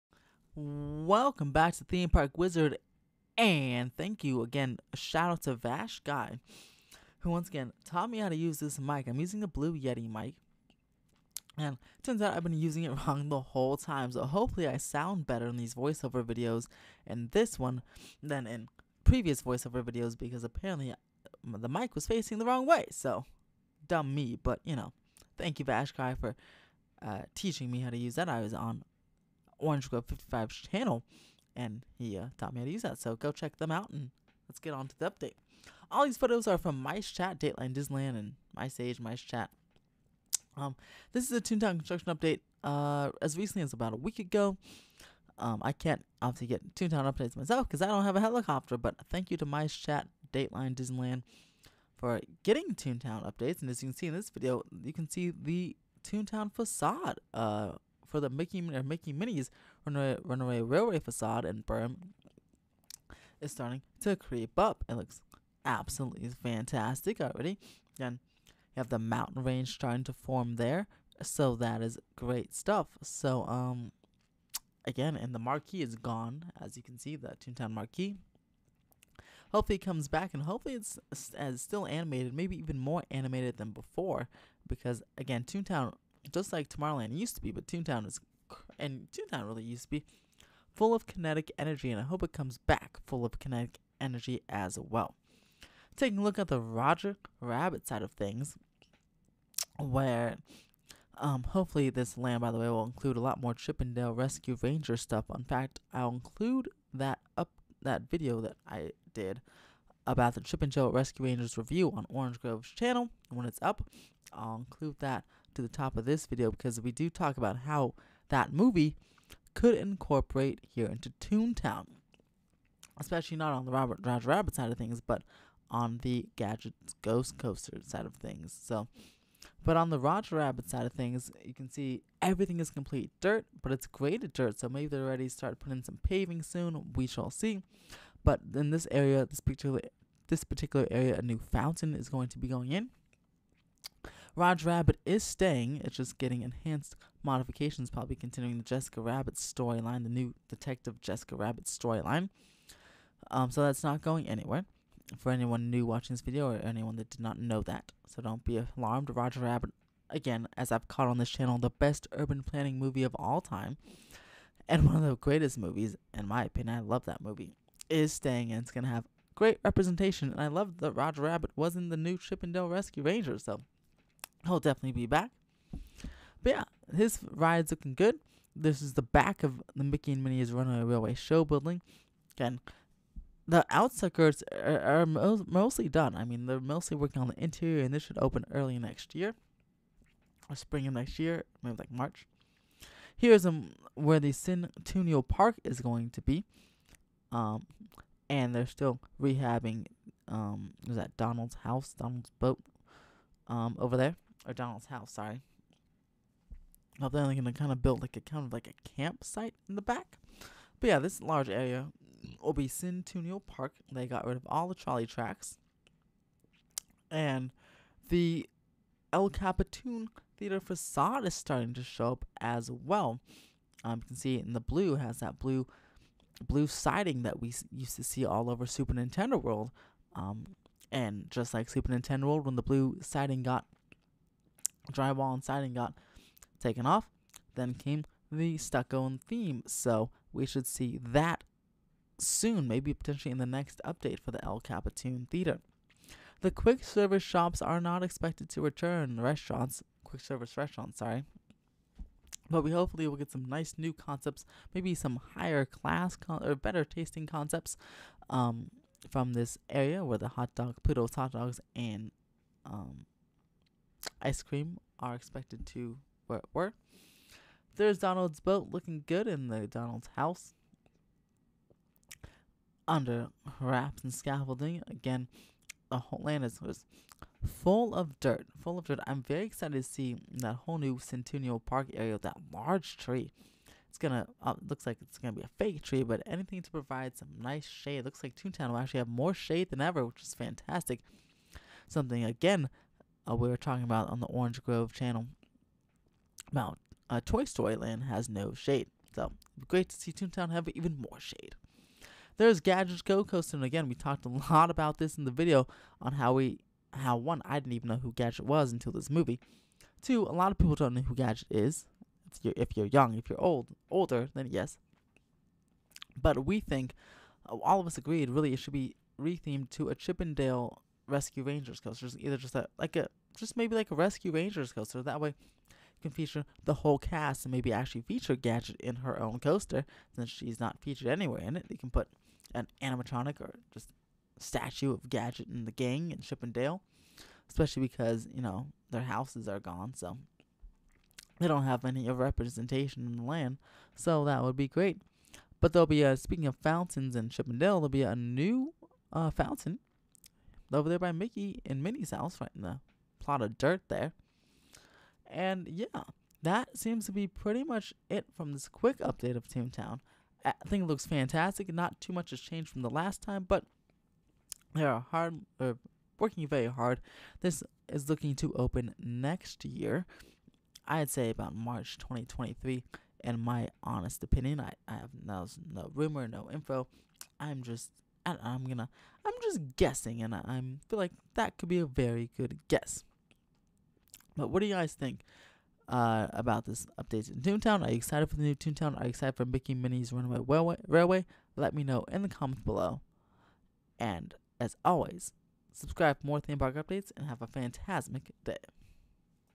Toontown Construction Update | Disneyland Park | Disneyland Resort
Thanks to a recent micechat update we have some great new areal photos of toontown at disneyland!